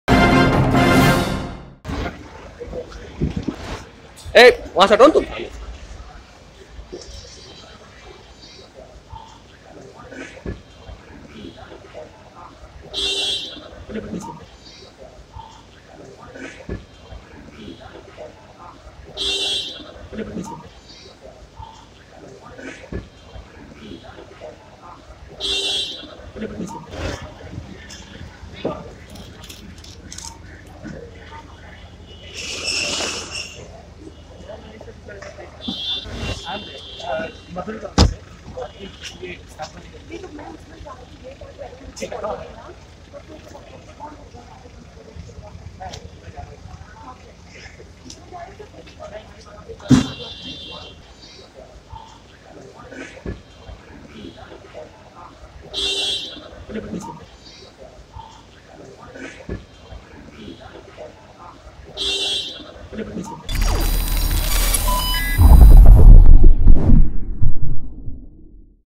Patna Airport पर Vidya Balan का जोरदार स्वागत | News4Nation
Patna में Vidya Balan का जोरदार स्वागत | News4Nation |